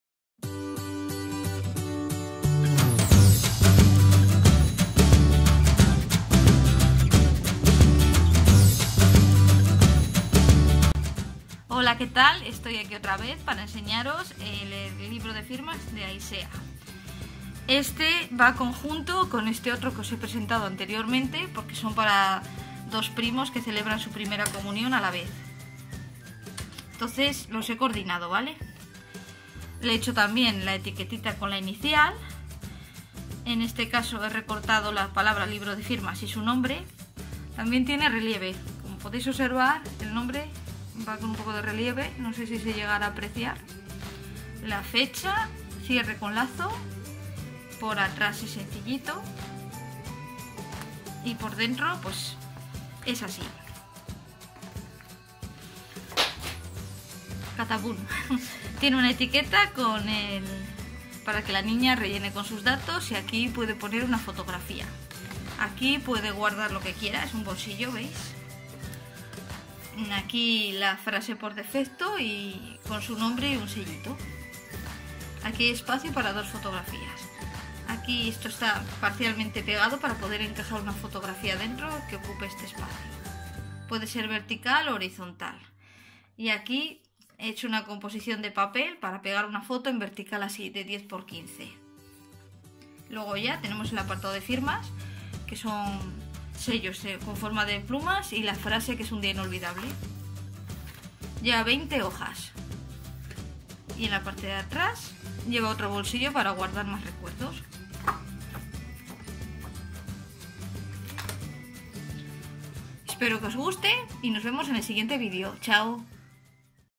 Libro de firmas comunión niña dayka 2017
Libro de firmas de primera comunión con papeles de dayka modelo del año 2017